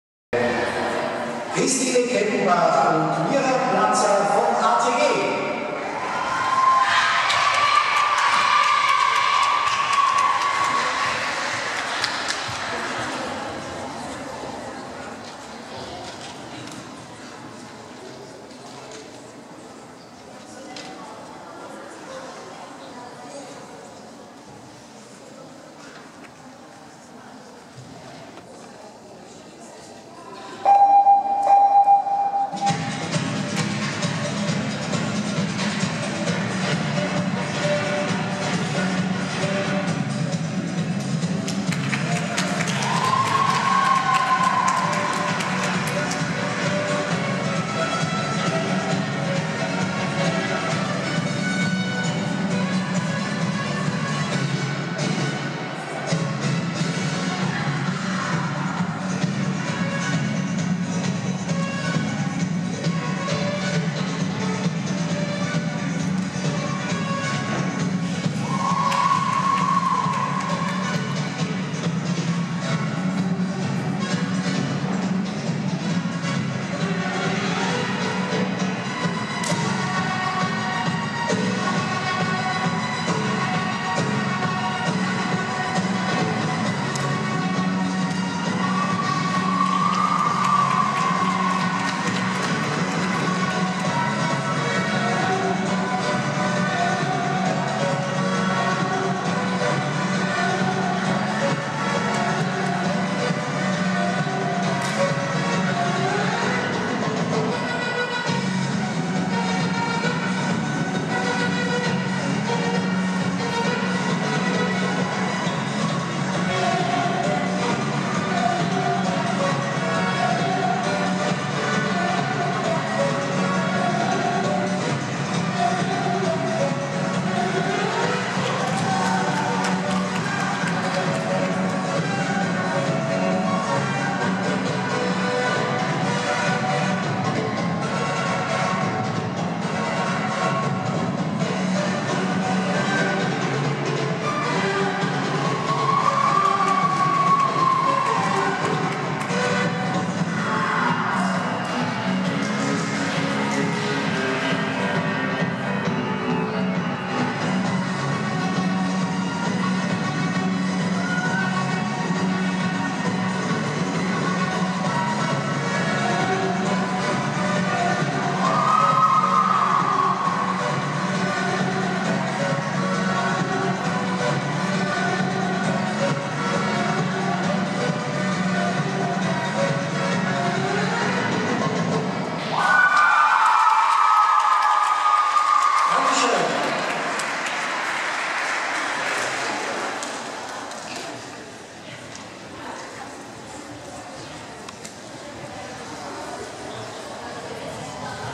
(11) Steirische LM 2015 - Offene B Paar - ATG
Christine Kettenbach und Mira Platzer
Steirische Landesmeisterschaften Sportakrobatik 2015
Jugend 2 und Offene B Küren vom Samstag 6.Juni 2015
im ATG Graz